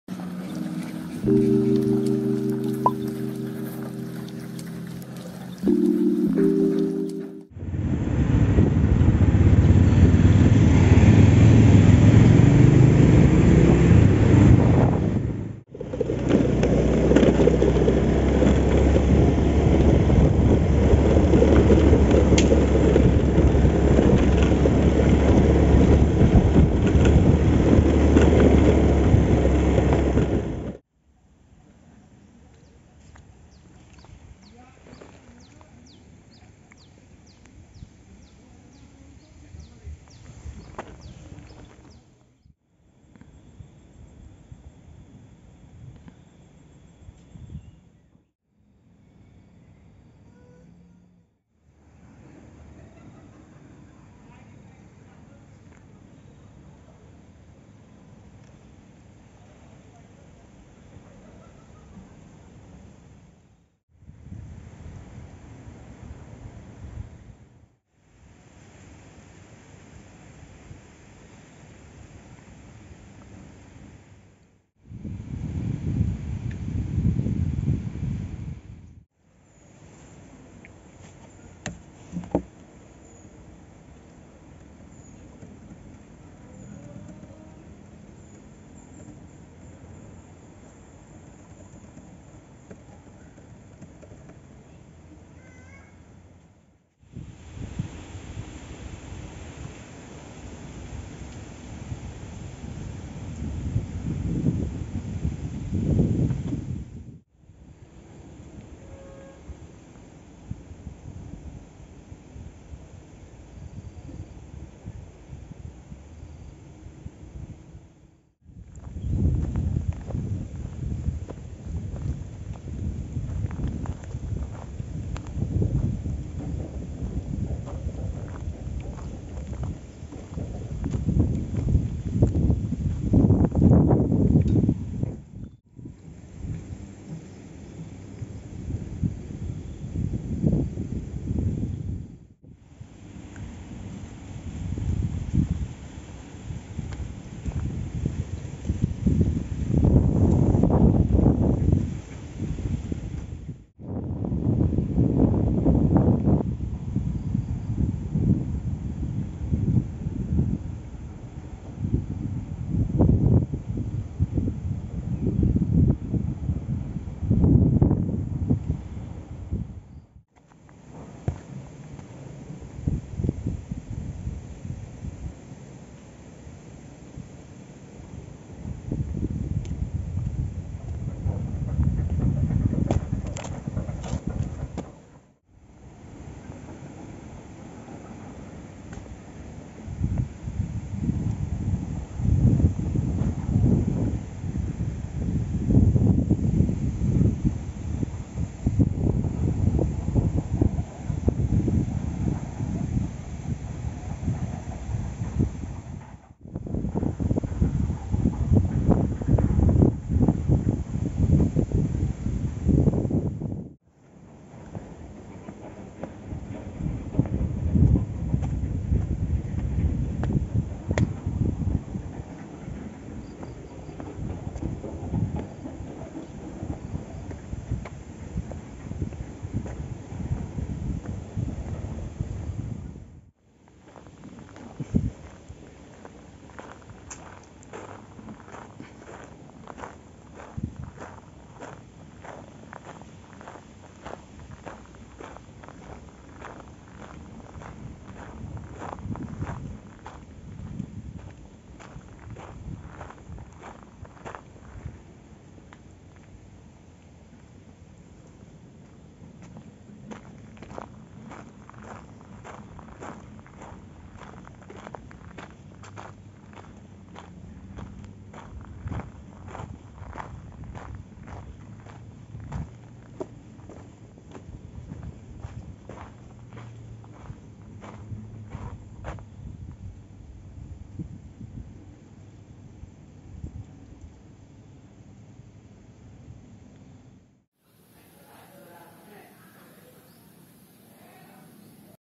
PEMANDANGAN PEGUNUNGAN - ASRI SEJUK SEPERTI DI KAMPUNG HALAMAN SEBAGIAN ORANG SAYANG KAAK CIAMIS
SAYANG KAAK
dusun cikatomas desa handapherang kecamatan cijeunjing kabupaten ciamis

kondisi disaat pandemi covid19 tempat wisata ini sangat terbatas aktifitasnya. sampai pengunjungpun skrg tidak diberlakukan banyak.